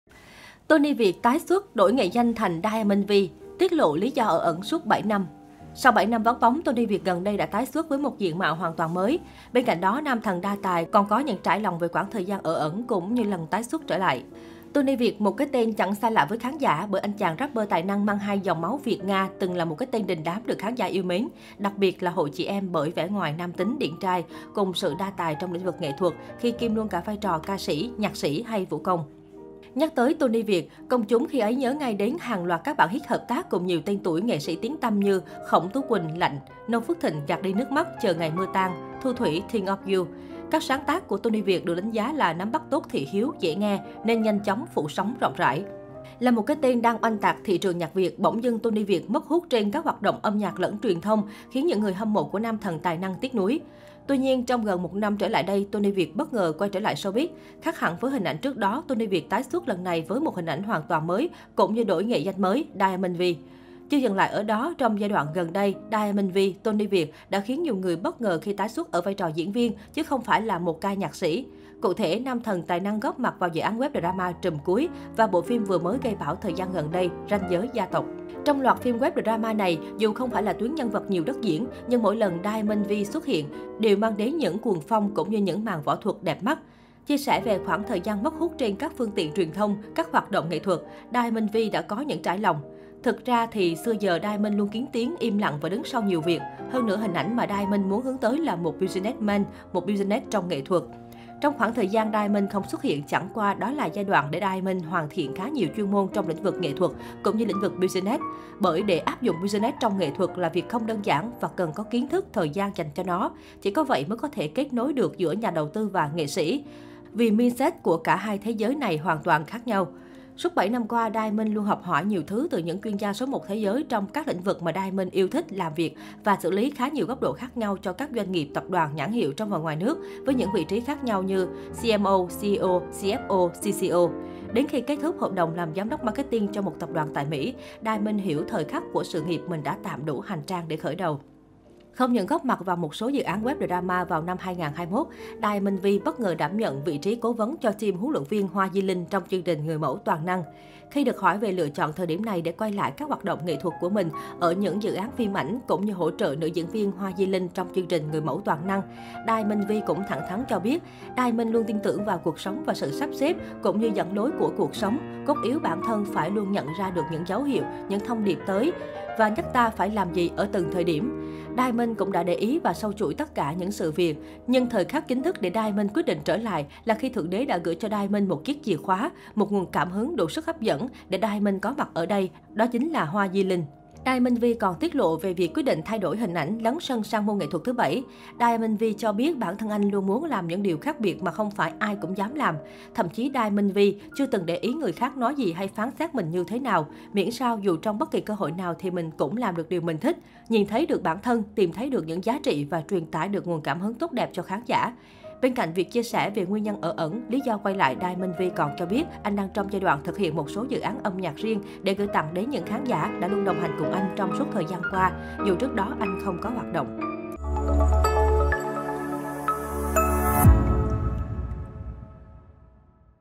Tonny Việt tái xuất, đổi nghệ danh thành Diamond V, tiết lộ lý do ở ẩn suốt 7 năm
Sau 7 năm vắng bóng, Tonny Việt gần đây đã tái xuất với một diện mạo hoàn toàn mới. Bên cạnh đó, “nam thần đa tài" còn có những trải lòng về quãng thời gian ở ẩn cũng như lần tài xuất trở lại.

Tonny Việt, một cái tên chẳng xa lạ với khán giả. Bởi, anh chàng Rapper tài năng mang 2 dòng máu Việt - Nga từng là cái tên đình đám được khán giả yêu mến , đặc biệt là hội chị em bởi vẻ ngoài nam tính, điển trai cùng sự sự đa tài trong lĩnh vực nghệ thuật khi kiêm luôn cả vai trò ca sĩ, nhạc sĩ hay vũ công.